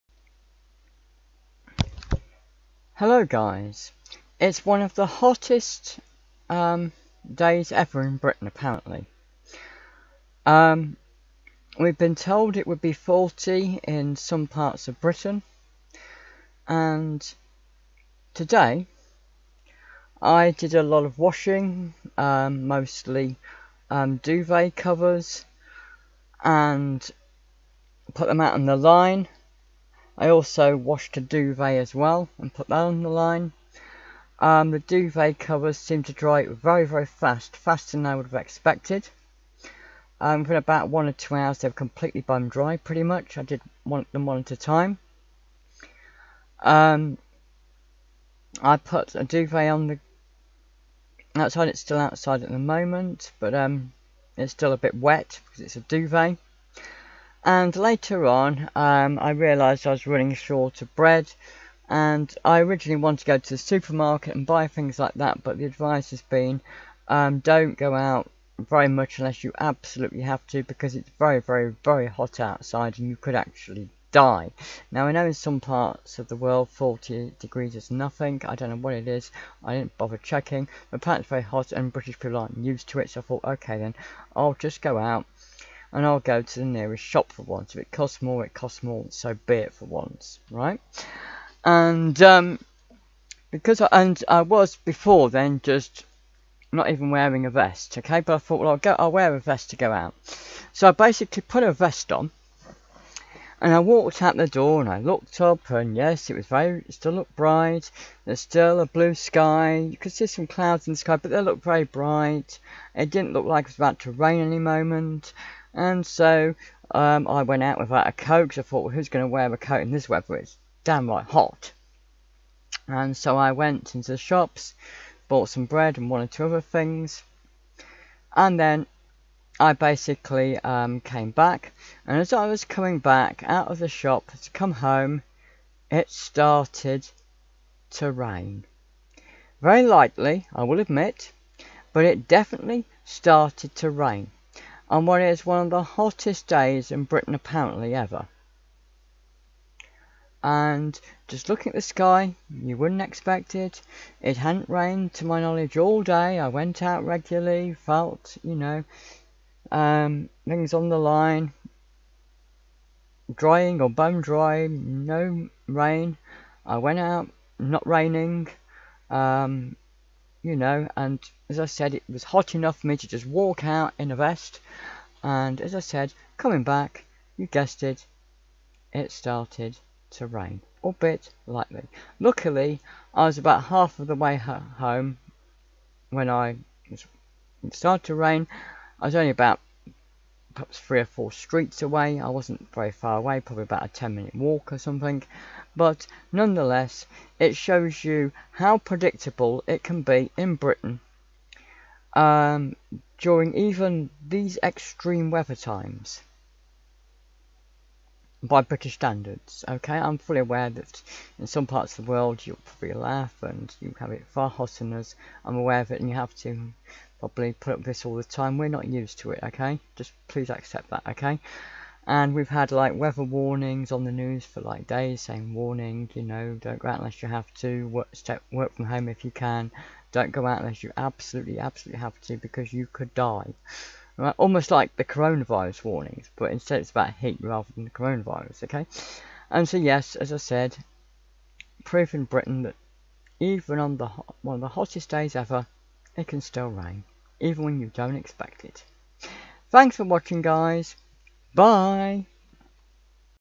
Cliff Discusses - British Problems - Rain When You Really Really Really Don't Expect It
It can rain absolutely anytime in Britain. Even when you don't expect it.
For days we were warned that today would be one of the hottest days in Britain ever with expected temperatures of 40C.
As it was so hot all day, and we had been warned previously for days that it would be extremely hot today, who would go out in a coat in boiling hot weather?
Would it be safe to nip out to a shop for half an hour to get some bread and other local food? What do you think? This is Britain.